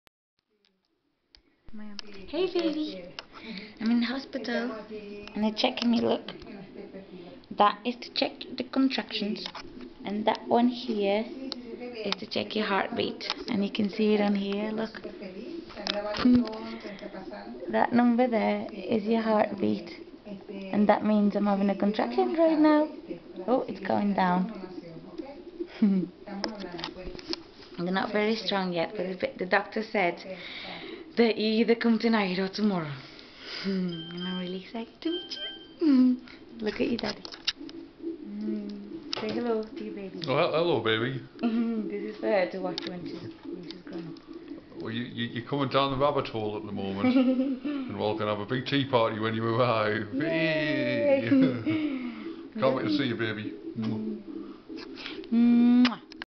Coming Down the Rabbit Hole!!!!
My waters had broke so I went over the hospital to get checked! I was SOoooooooooooooooo excited! A lil anxious too! BUT SO EXCITED! :D